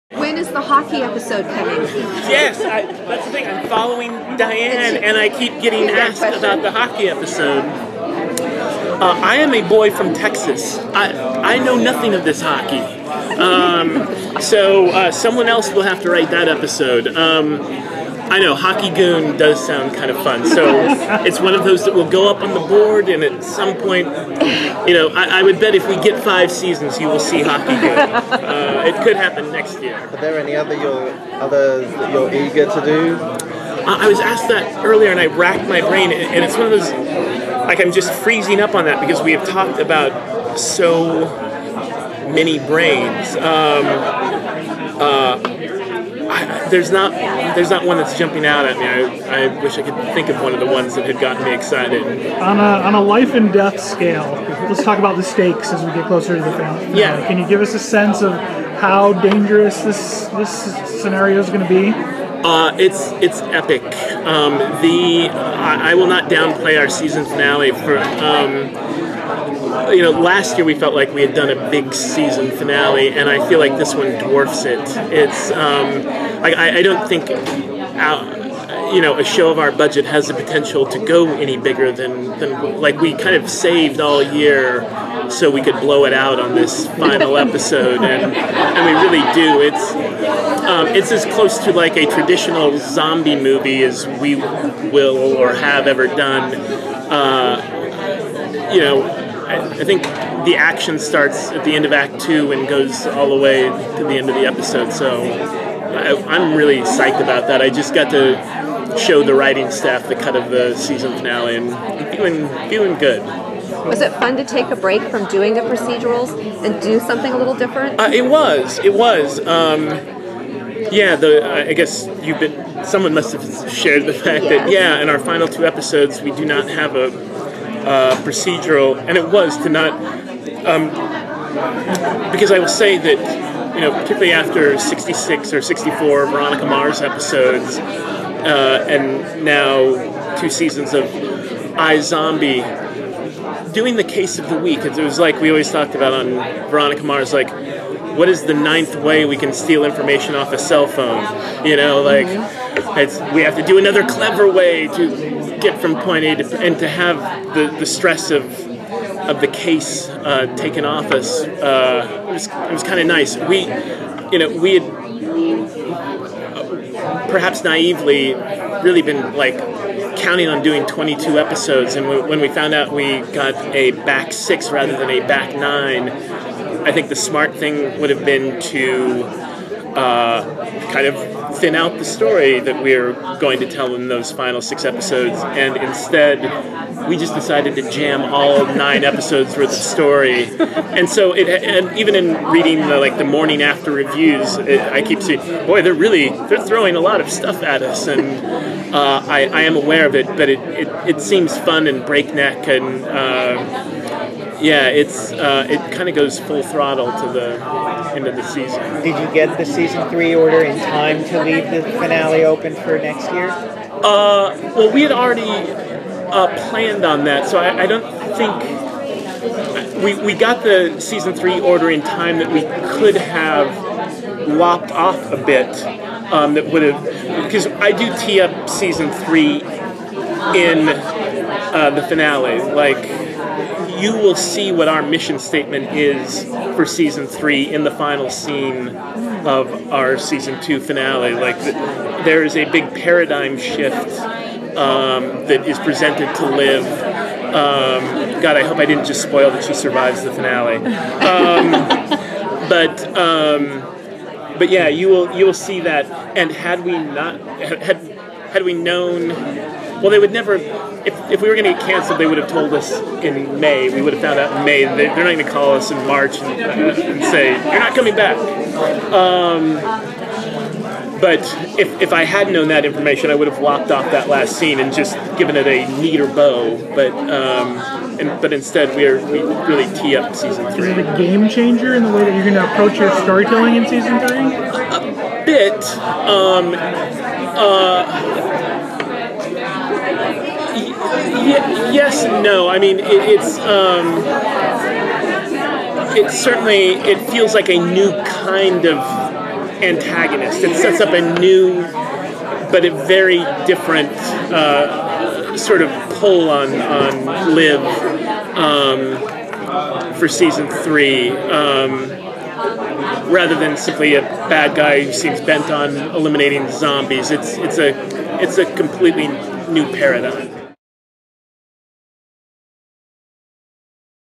iZombie - Rob Thomas Interview, Season 2 (WonderCon)
iZombie executive producer Rob Thomas interview on season 2 and what fans of the series can expect from the big finale. The interview took place at the 2016 Wonder Con in Los Angeles, Ca.